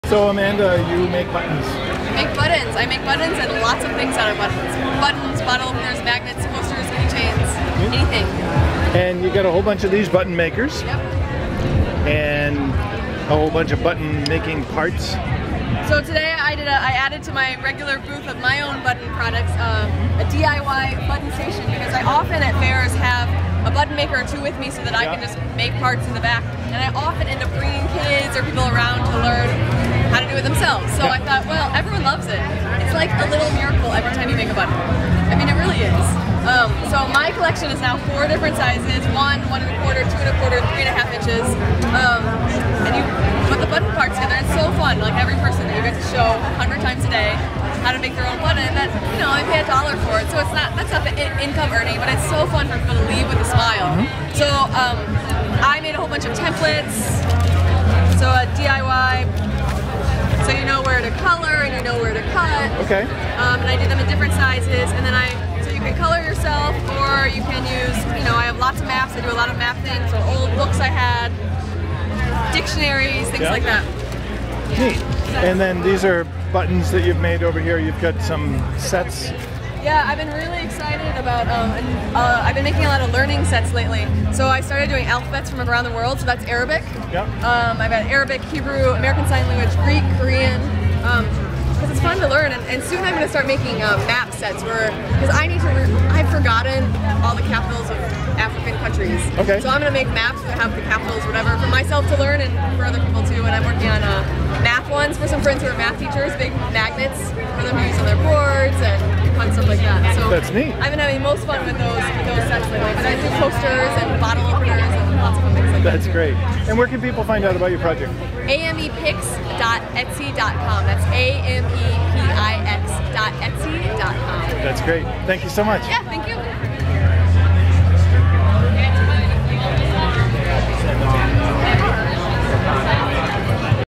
Amanda makes buttons!
At Detroit Maker Faire, I met up with Amanda, who had a great collection of buttons, button makers and supplies to make your own custom button.